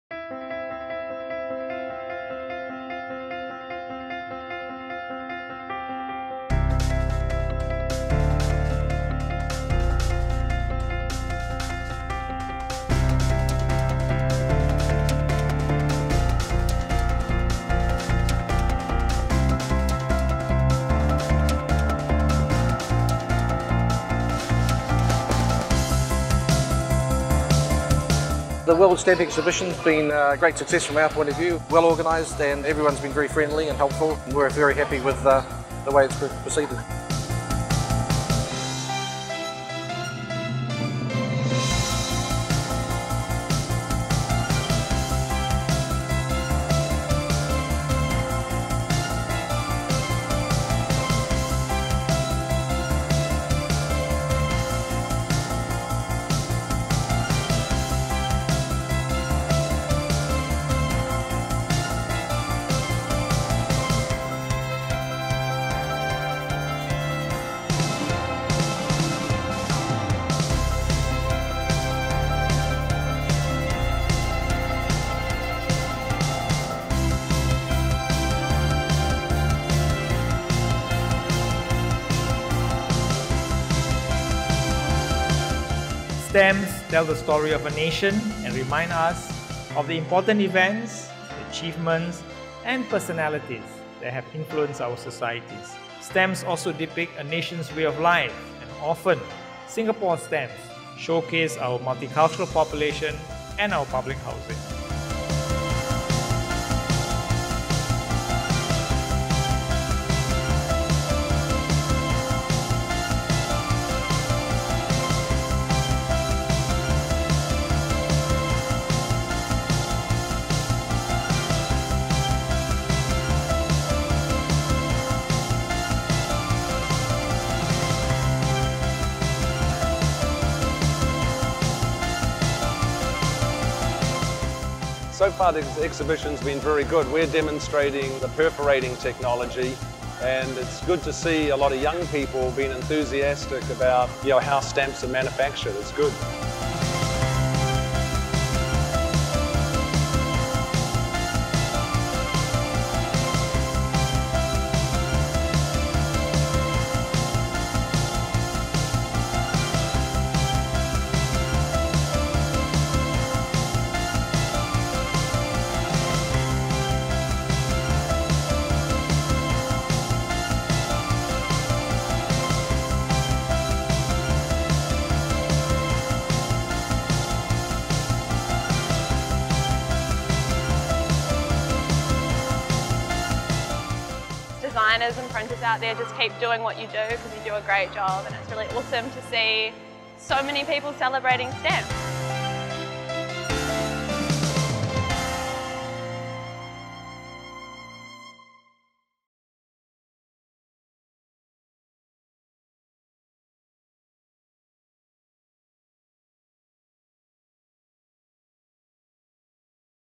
SINGAPORE 2015 World Stamp Exhibition Highlights
held at Sands Expo & Convention Centre, 14-19 Aug 2015